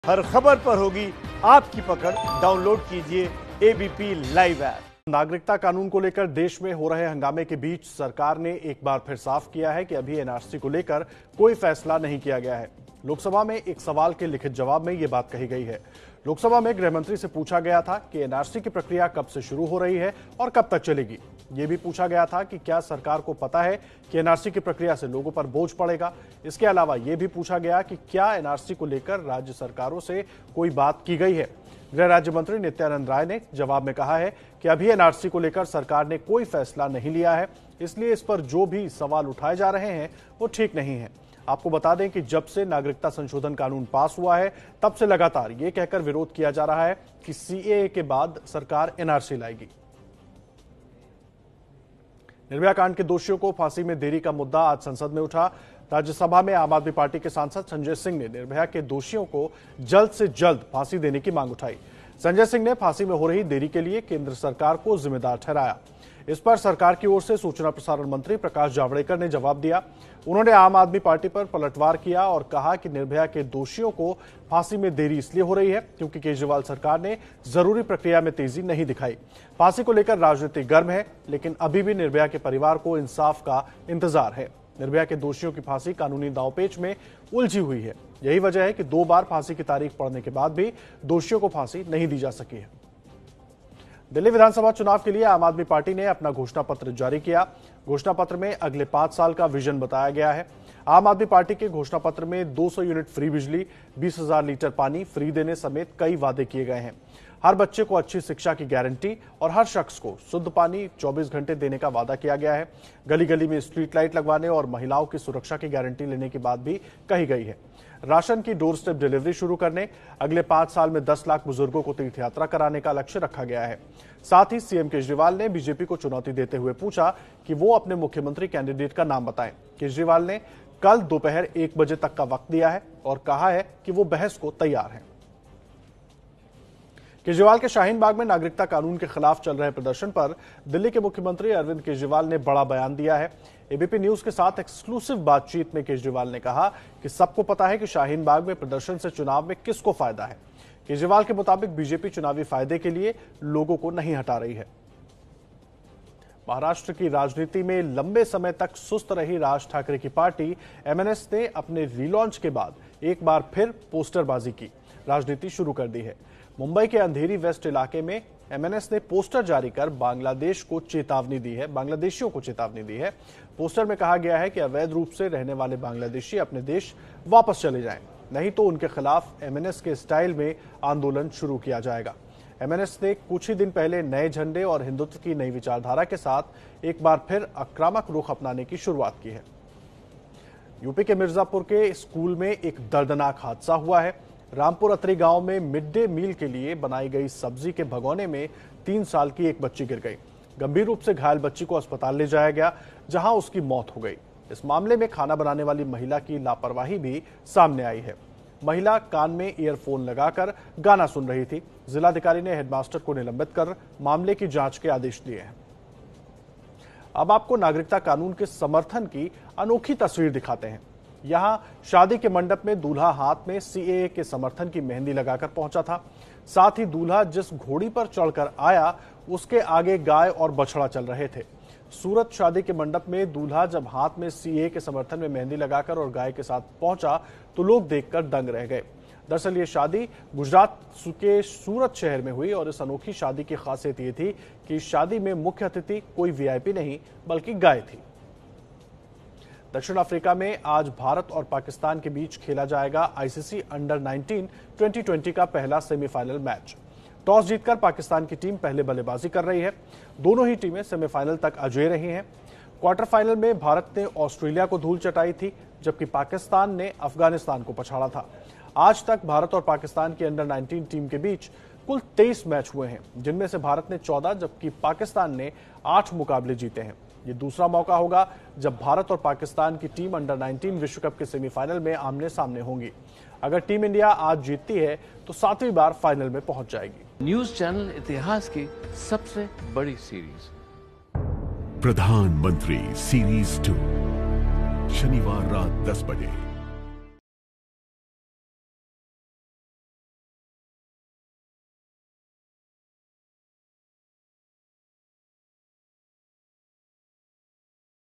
लोकसभा में NRC पर सरकार का बड़ा बयान | ABP News Hindi
NRC पर सरकार का बड़ा बयान-NRC तैयार करने पर अबतक फैसला नहीं, लोकसभा में सरकार ने लिखित में दिया जवाब.